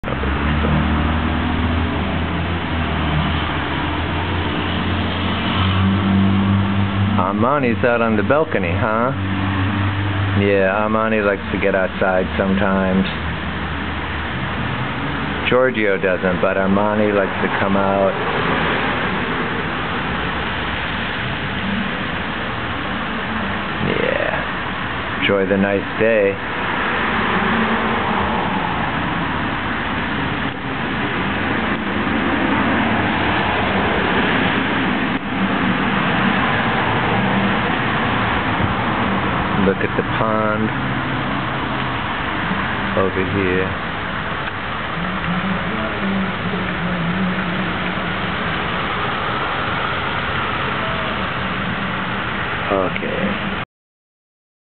Armani on the balcony
Habibi's cat, Armani, loves to go out on the balcony in nice weather.